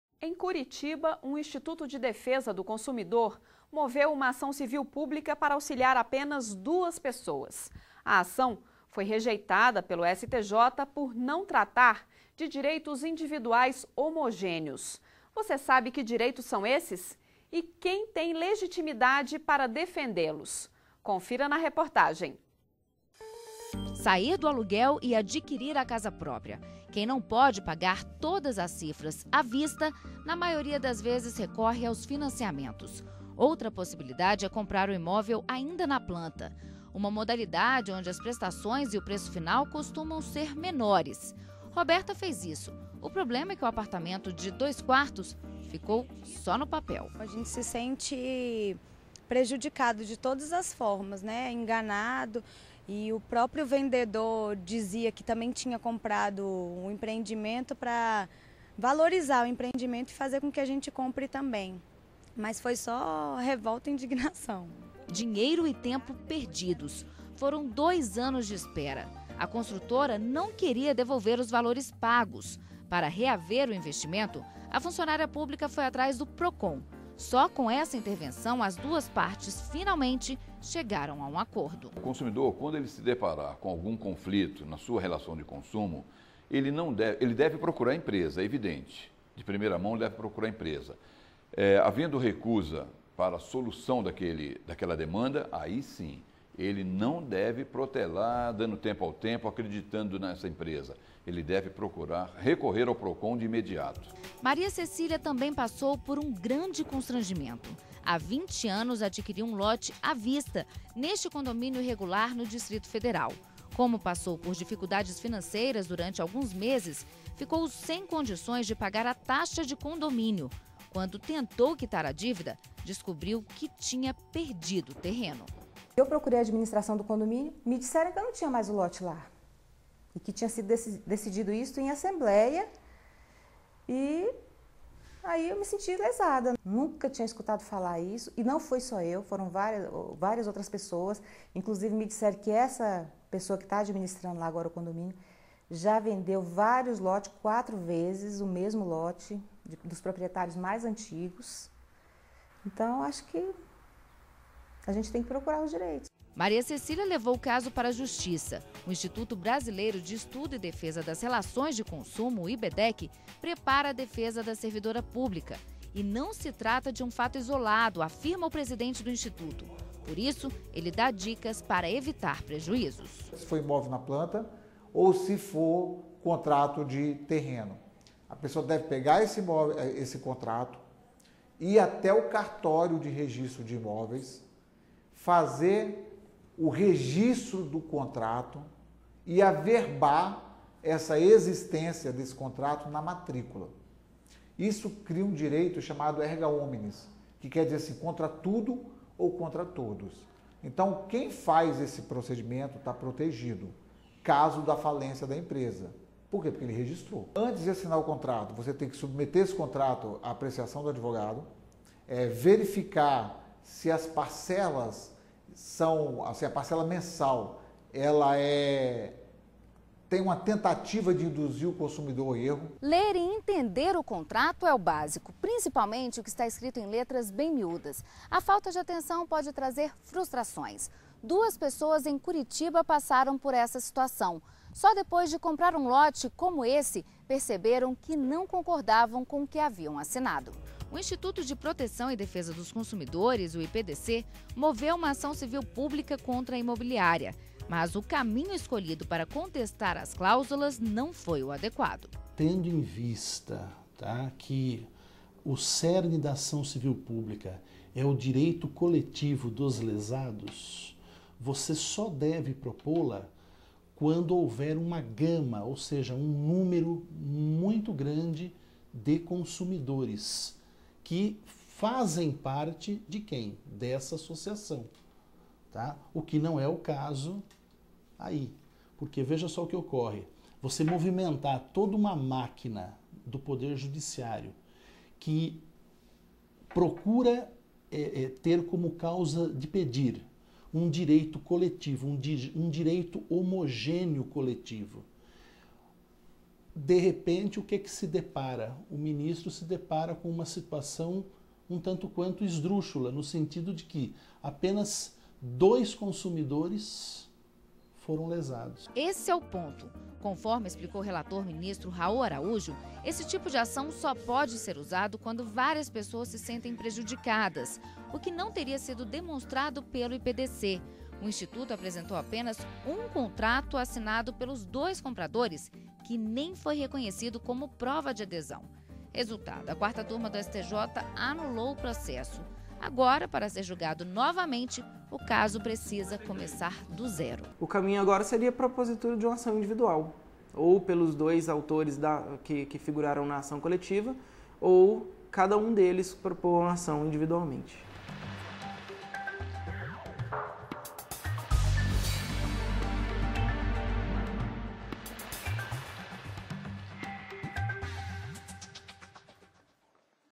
Entenda a função de uma ação civil pública - STJ Cidadão
Entenda a função de uma ação civil pública